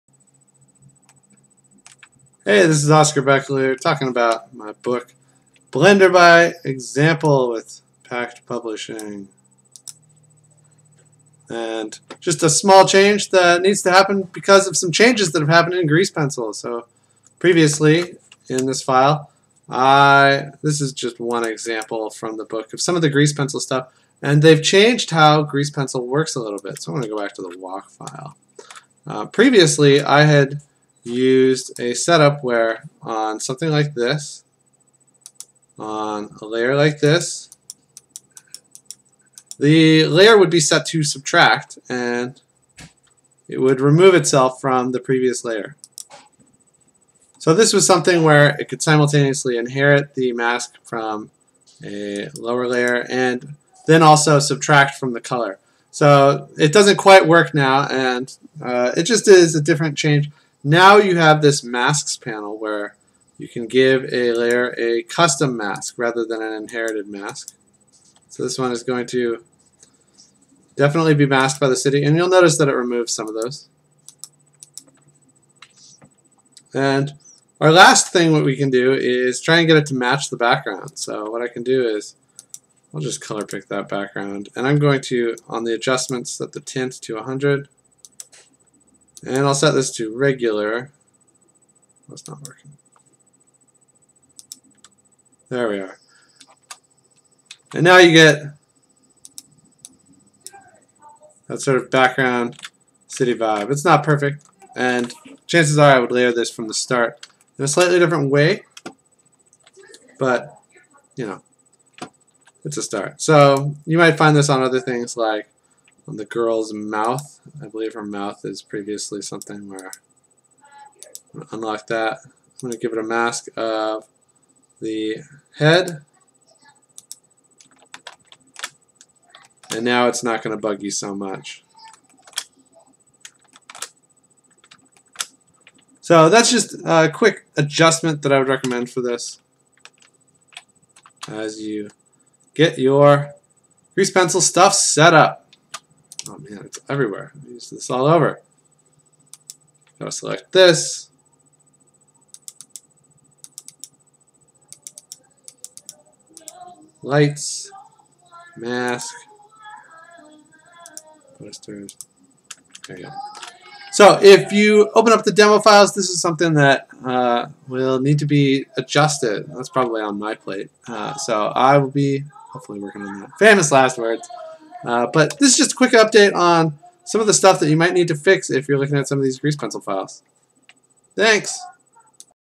Blender 3D By Example updates to Grease Pencil
Blender keeps getting more awesome!!! Here's a small window into some changes on how Grease Pencil layer masks work, and how you'd adjust the project files to work with the newer 2.9 setup.